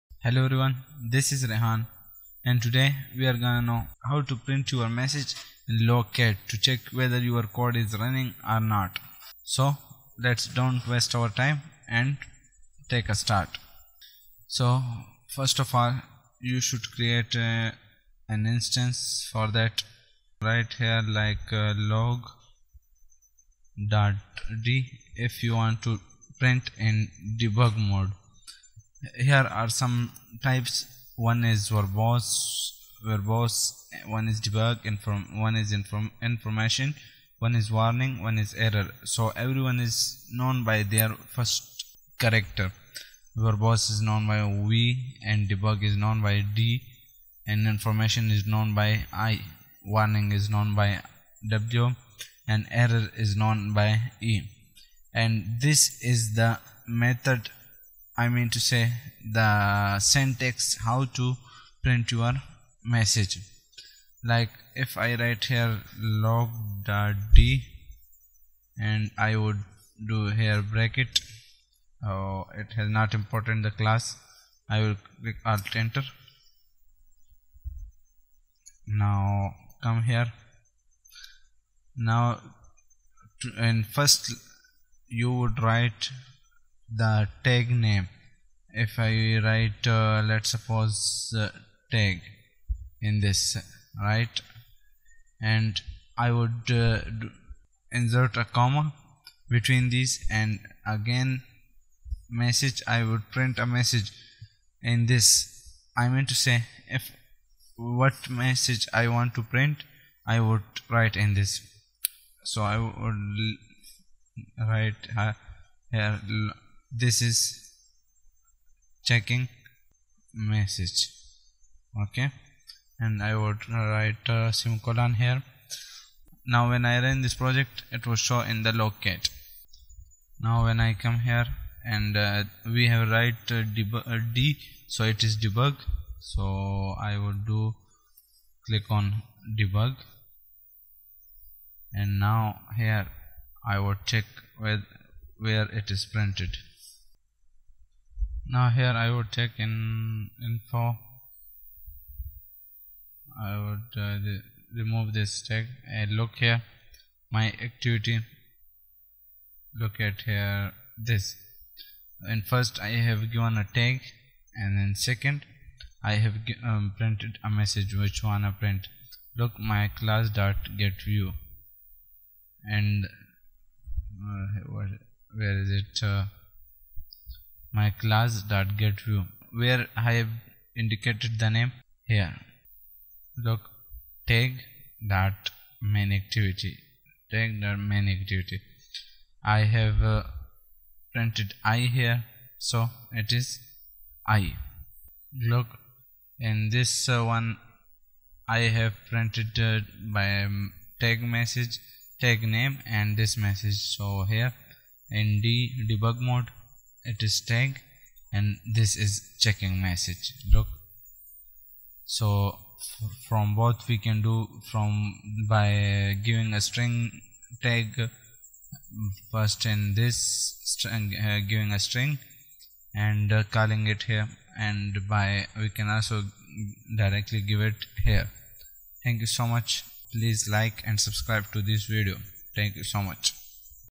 How to print Log Message in Logcat in Android Studio
In This Video, I have shown how to print Log messages in Logcat in Android Studio